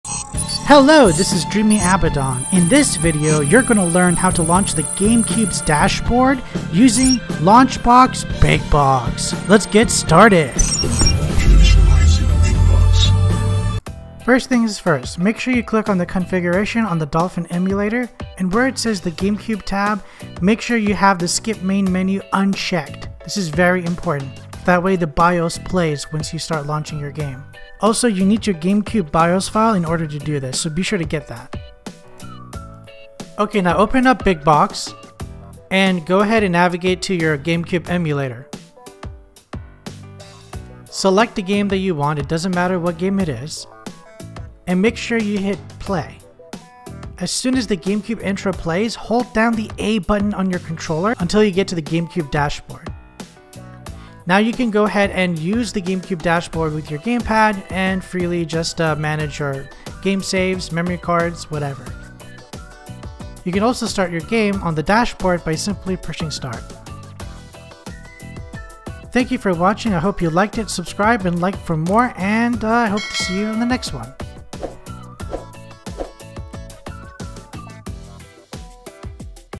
How to Launch GameCube Dashboard with BigBox
Have you ever wanted to launch the GameCube Dashboard using launchbox / BigBox? In this short tutorial you will learn how to do just that!